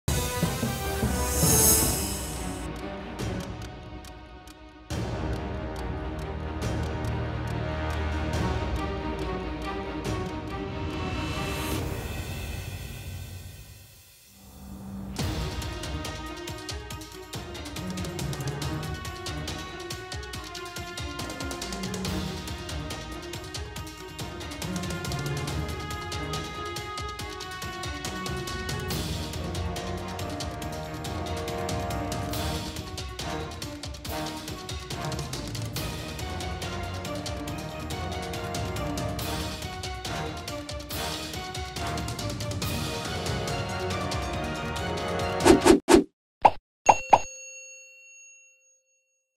Online रिश्तों से सावधान | दोस्ती, शादी और दुष्कर्म
Shadi.com के जरिये करता था लड़कियों से दोस्ती, बनता था शारीरिक संबंध